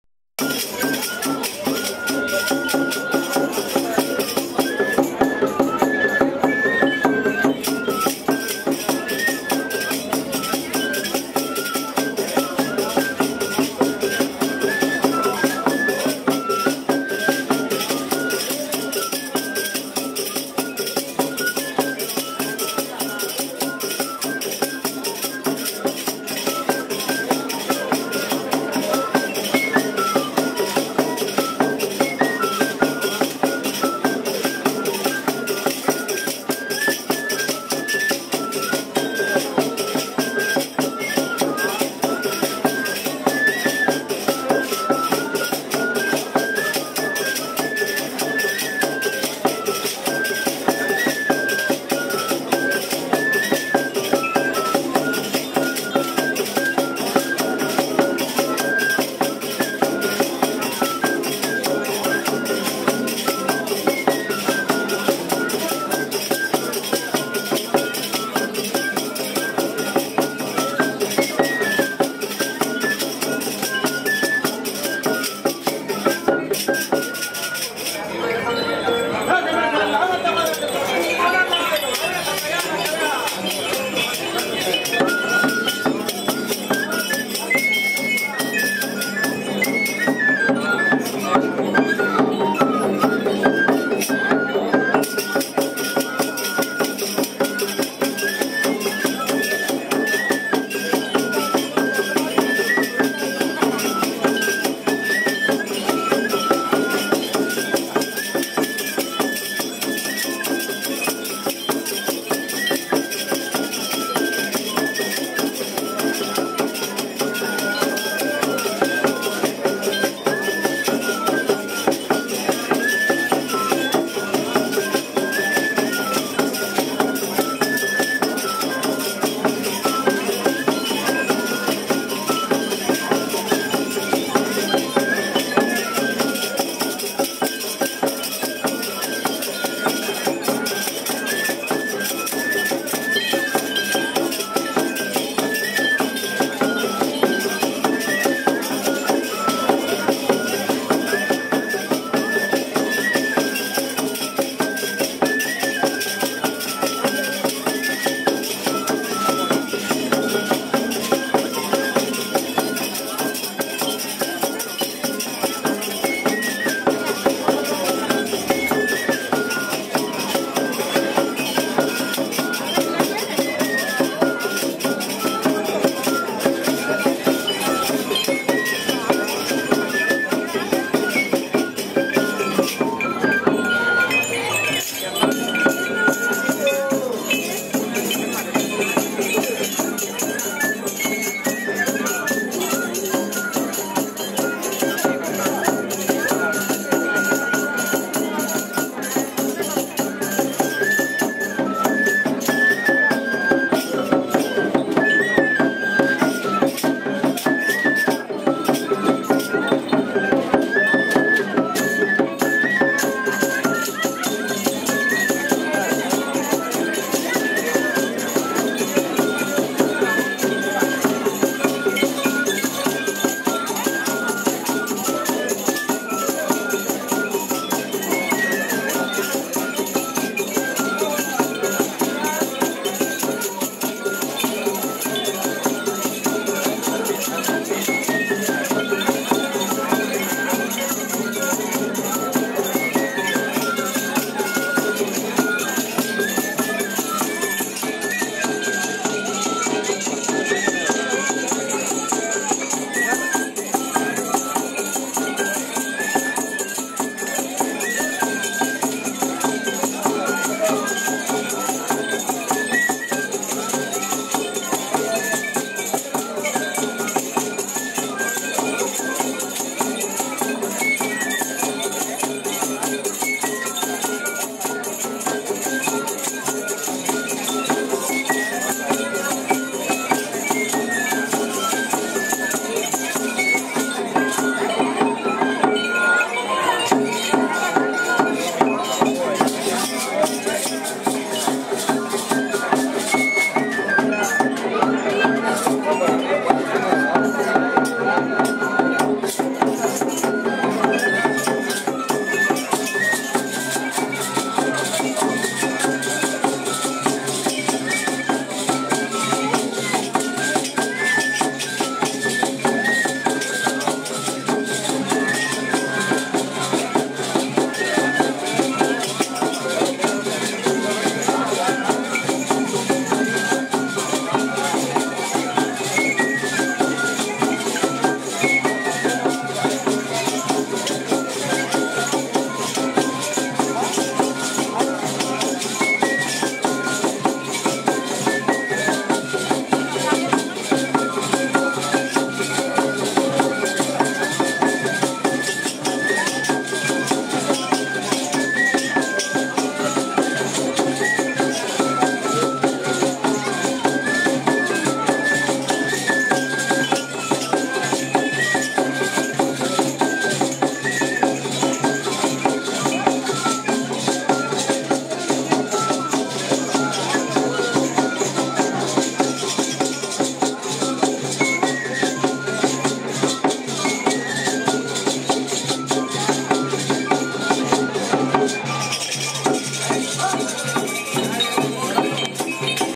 Domingo de Resurreccion Parte 1
Pascolas Bailando en la Ramada de la Fiesta
San Miguel Zapotitlan